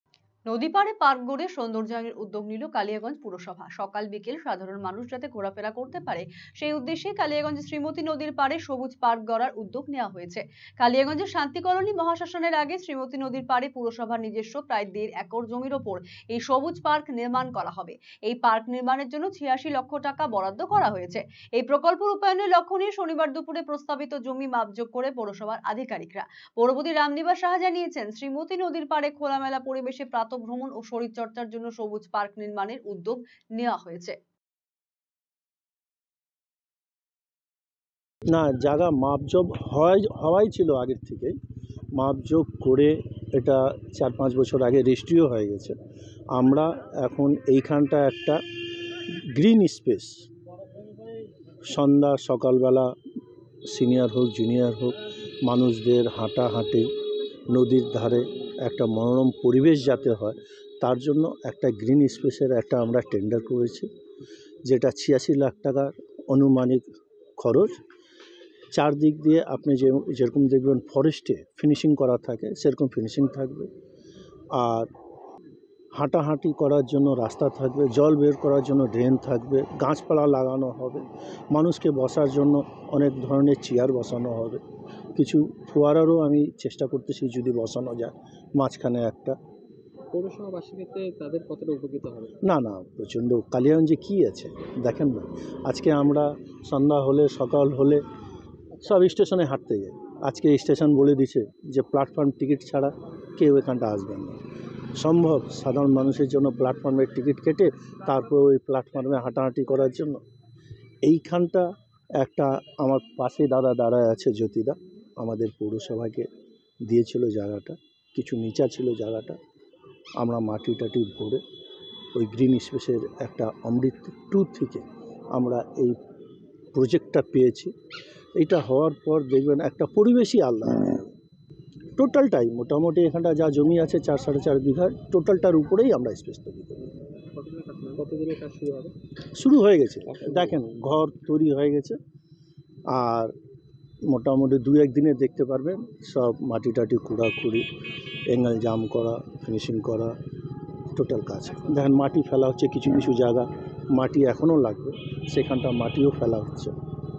Kaliyaganj News | নদীপাড়ে পার্ক গড়ার উদ্যোগ পুরসভার | Rctv Sangbad
সংক্ষেপে খবরের বিষয়ঃ  নদী পাড়ে পার্ক গড়ে সৌন্দর্যায়নের উদ্যোগ নিল কালিয়াগঞ্জ পুরসভা। সকাল বিকেল সাধারণ মানুষ যাতে ঘোরাফেরা করতে পারে সেই উদ্দেশ্যেই কালিয়াগঞ্জের শ্রীমতি নদীর পাড়ে সবুজ পার্ক গড়ার উদ্যোগ নেওয়া হয়েছে।